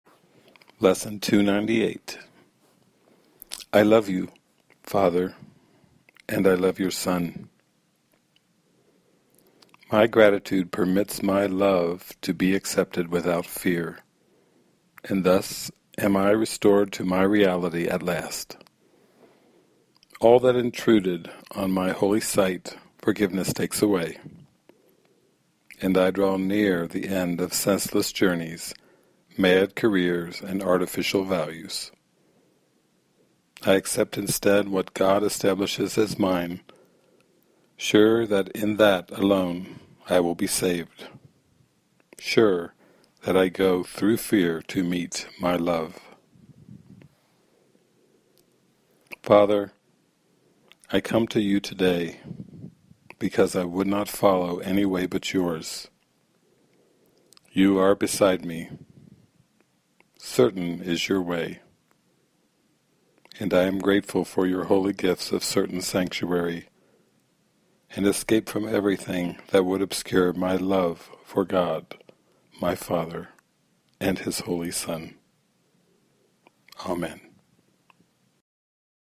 ACIM Lesson - 298 "I love You, Father, and I love Your Son" by David Hoffmeister
Practice and live the mystical teachings of Jesus assisted by online courses, online retreats, and an online Tribe of Christ community: David Hoffmeister, who speaks from the Awakened Mind, continues a journey through the spiritual classic A Course In Miracles by reading from The Workbook, Lesson 298 "I love You, Father, and I love Your Son".